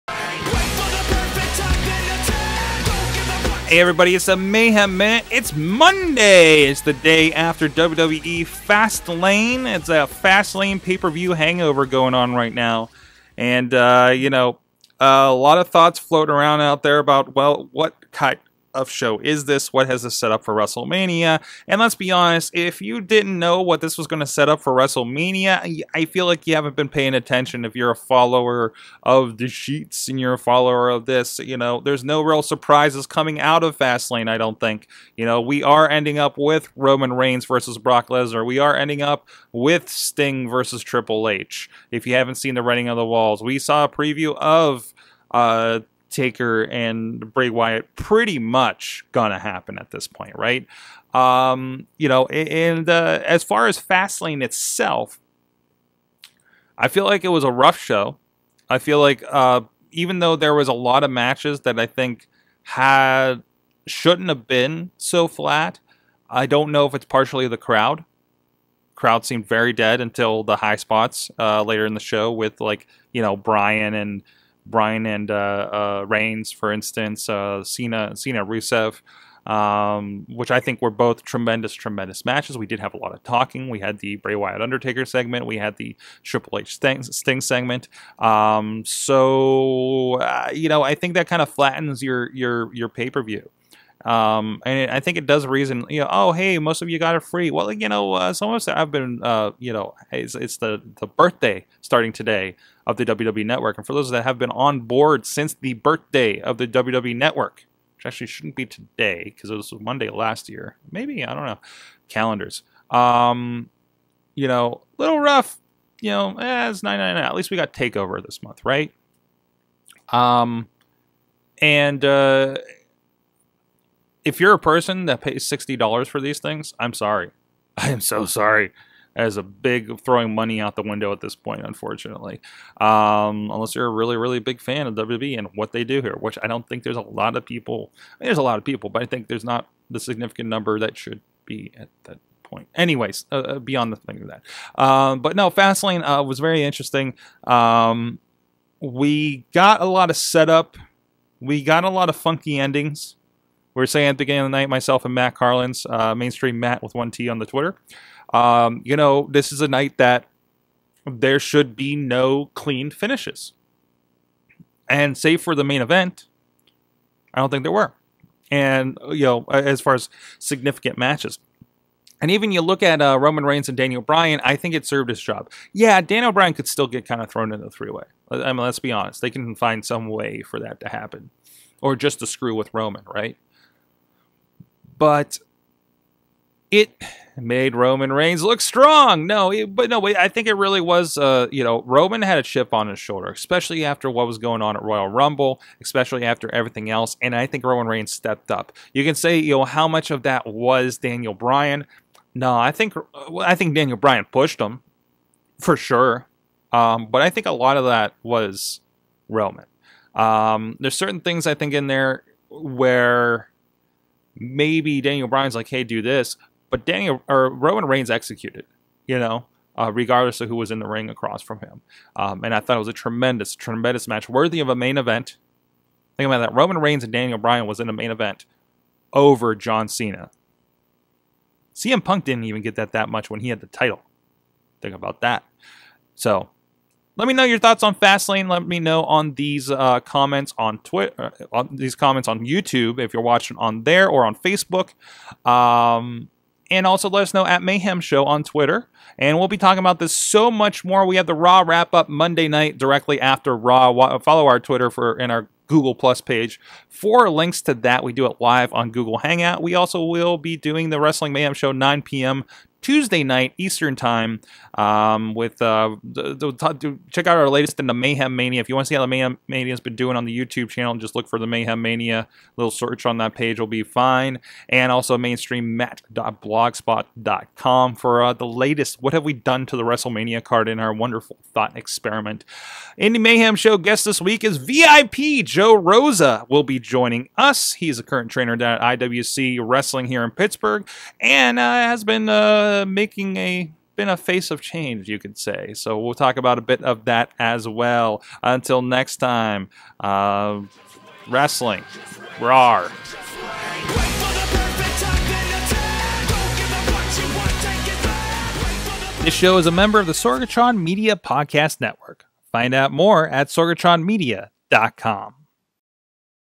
Mayhem Minute: WWE Fast Lane Review
Sorg gives some quick thoughts on WWE Fast Lane.  Did Roman Reigns deliver?  Was this a great "free" show?  What about those that still pay?  What was with that crowd?